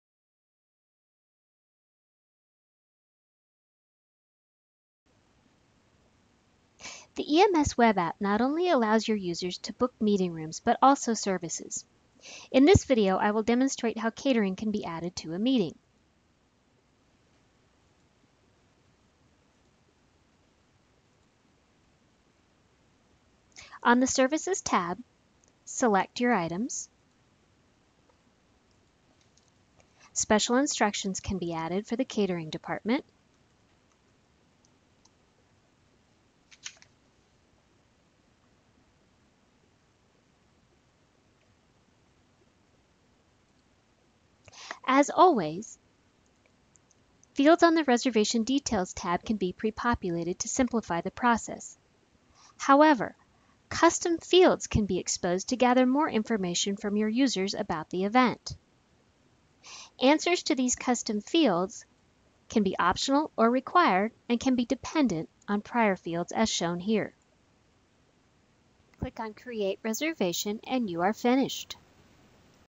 Web App: Book a Meeting with Catering
Book a meeting room, catering, UDF and dependent UDF